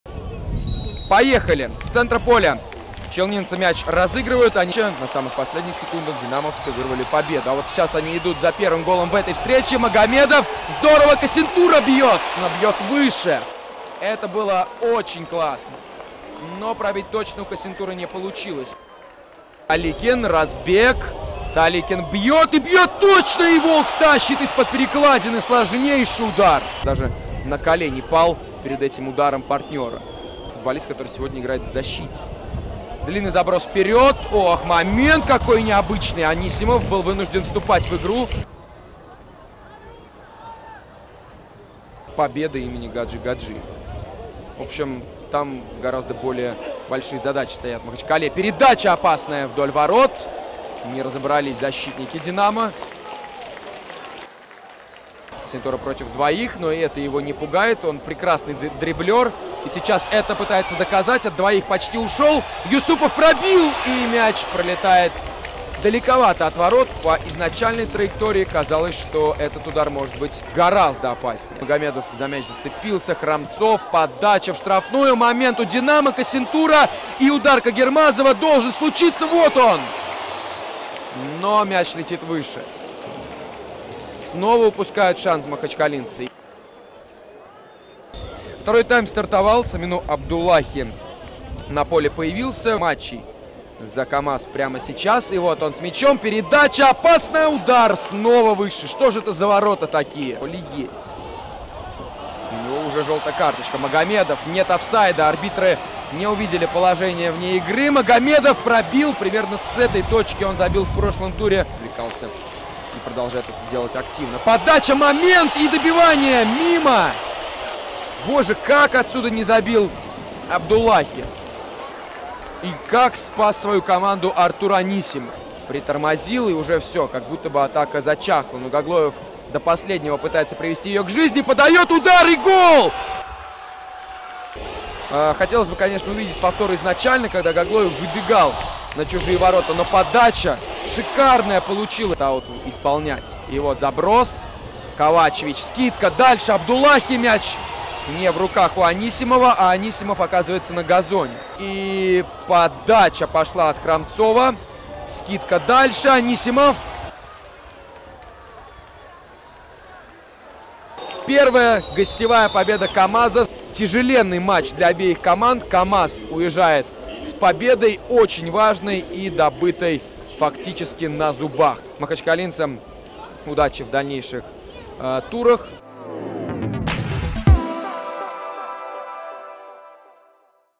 «Динамо» (Махачкала) – «КАМАЗ» (Набережные Челны) – 0:1
4-й тур МЕЛБЕТ – Первой лиги 2023/2024

6 августа 2023. Махачкала. Стадион «Труд»